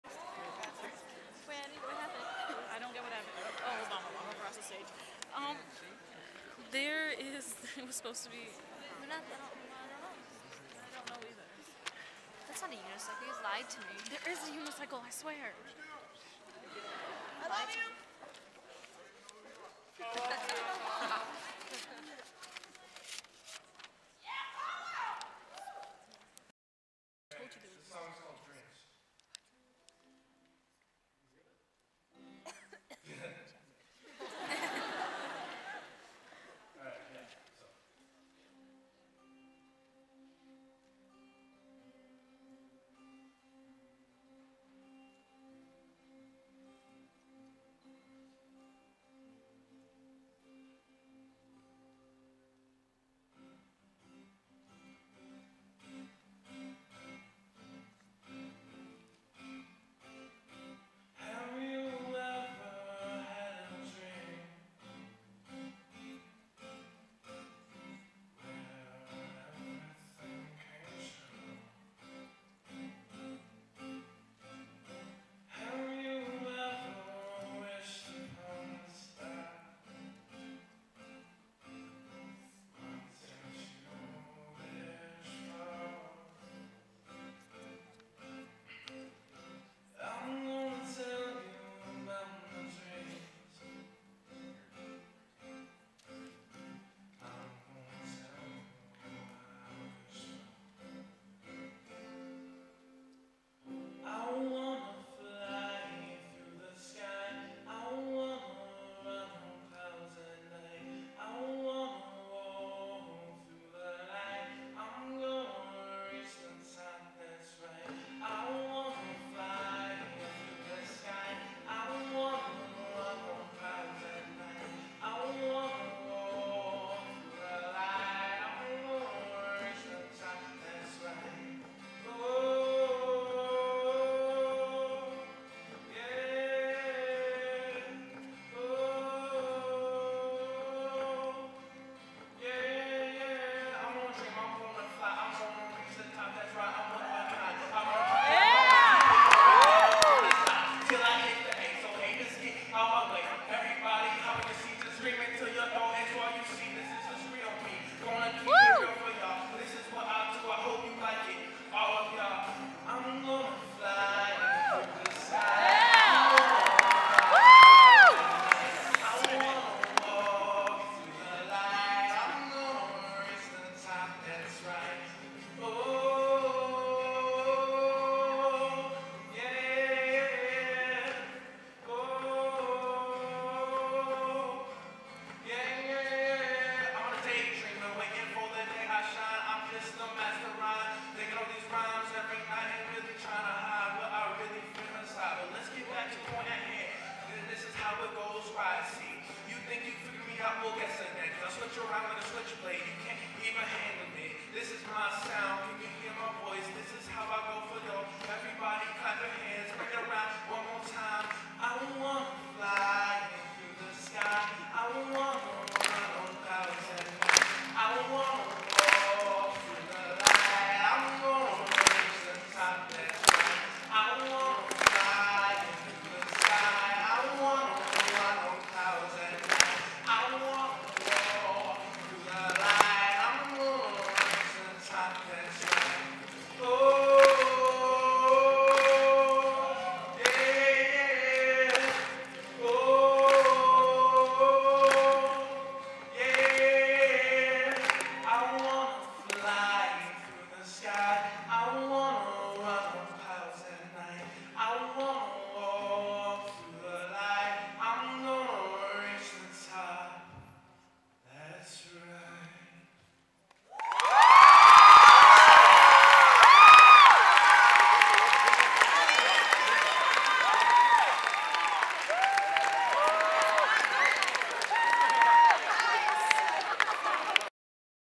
Lake Zurich High School Talent Show 2011 - Apollo Millon
Apollo Millon preforms in the LZHS Charity bash Talent show!

Please support the Crohn's & Colitis Foundation!
*also a special thankyou to the LZHS student Council and the Technical Crew who made this show possible.